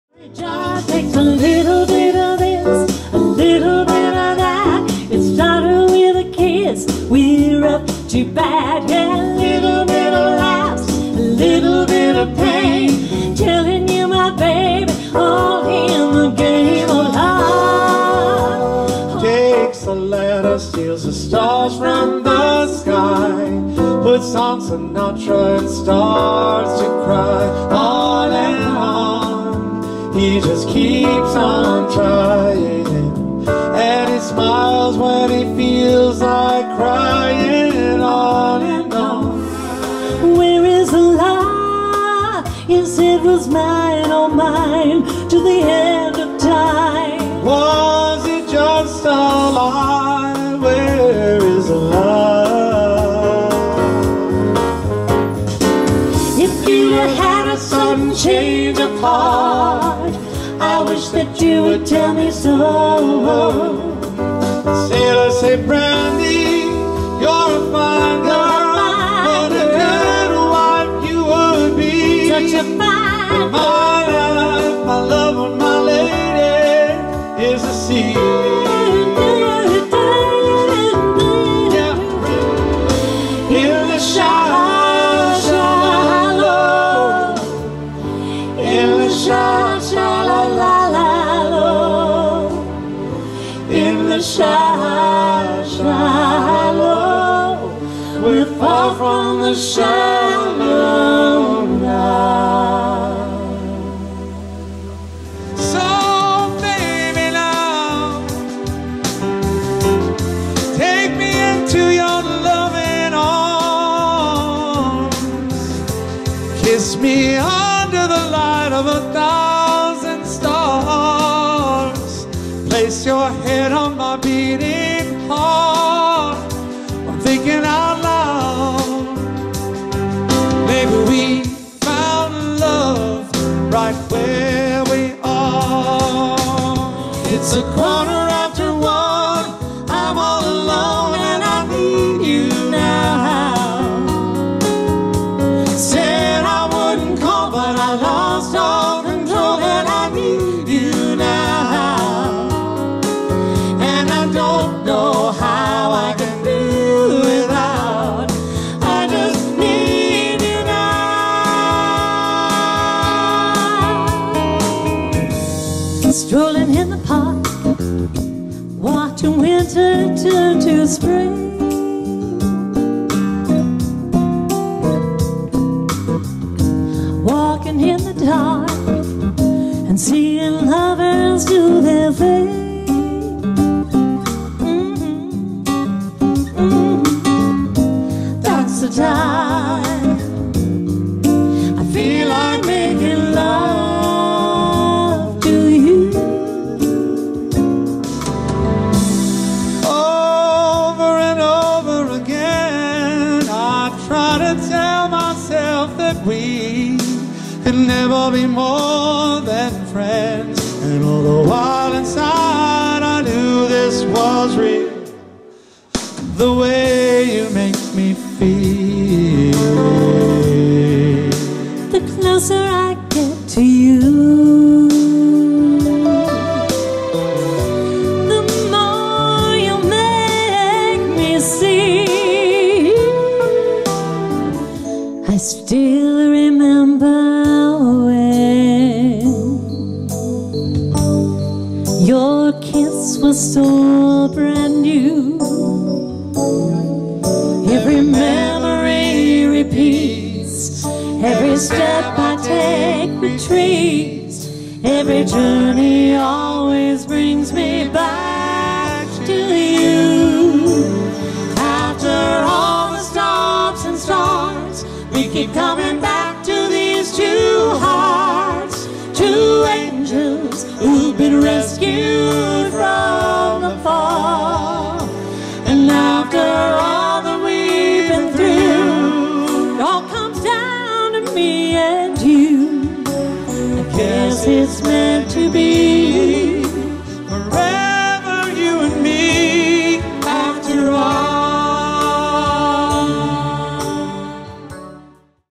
Double Take - Janet Hammer & Nathan Fry - 2019
Double Take is San Diego's premiere vocal/keyboard duo.  Nathan Fry & Janet Hammer have been playing together for 30 years.  This video is live from Orfila Winery and features some of their mellow/Yacht Rock/pop vibe tunes.